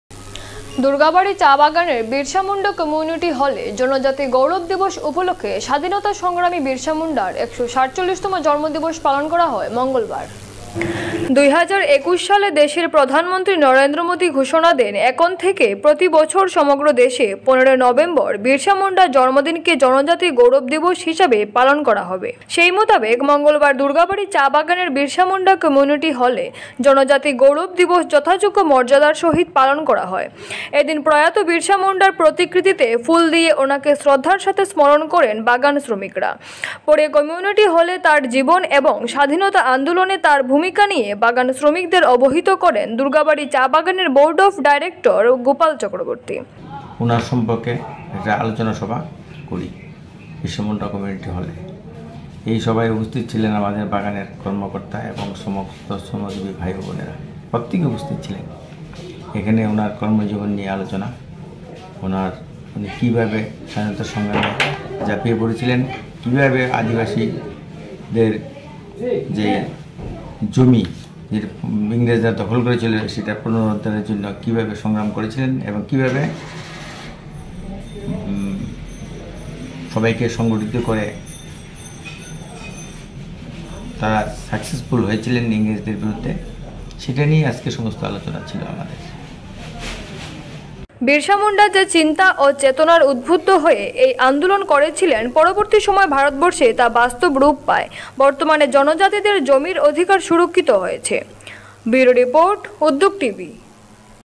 স্বাধীনতা সংগ্রামি বিরসা মুন্ডার ১৪৭ তম জন্ম দিবস পালন করা হয়
দুর্গা বাড়ি চা বাগানের  বিরসা মুন্ডা কমিউনিটি হলে জনজাতি গৌরব দিবস উপলক্ষে স্বাধীনতা সংগ্রামি বিরসা মুন্ডার ১৪৭ তম জন্ম দিবস পালন করা হয় মঙ্গলবার